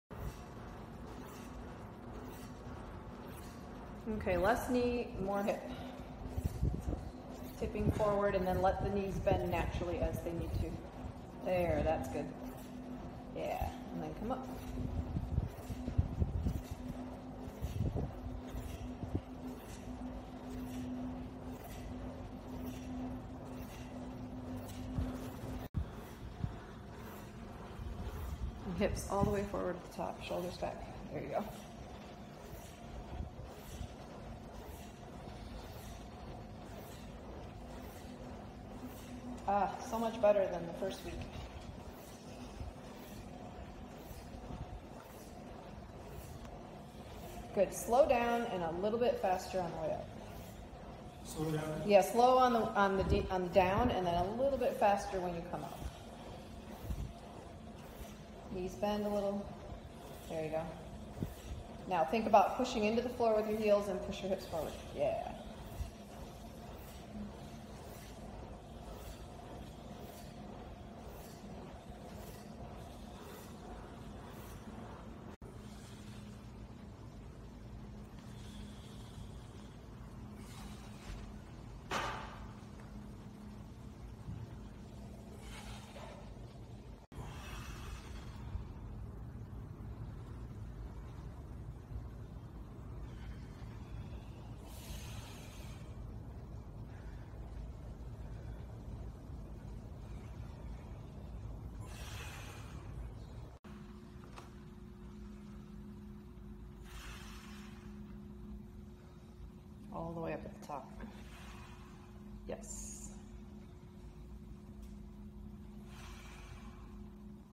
Hip Hinge Progression - Proper Form for Deadlifts and Picking Up Heavy Things!
I assess a client's hinge pattern in our first ever session together, and I quickly get a sense for whether or not the movement comes naturally to them.

It's one of those where people either naturally do it well, or they struggle to put the pieces together. It's a very technical lift.

And it's one of my non-negotiables. I'm going to work with clients until their hinge pattern is strong and smooth as butter. We use the stick to groove the pattern. We use light loads until form is nailed in.

However, a weight that is TOO light really doesn't help the movement "click." Getting ready to lift something heavy makes you truly understand why you need to brace your core and use good form, so grab something just heavy enough to make your brain think, "careful!"

Once clients can do this movement well, I know they are less likely to strain their backs when they move rocks in their yard or haul a full basket of laundry or pick up their toddler.

I have several principles I abide by in my training style, and the main one is safety. Safety while exercising with me, safety while they're moving about in their daily lives, and safety in proper exercise assignment & progression.

In this video look at the fabulous progression of my client's hip hinge pattern! Now, he can hip hinge like a pro!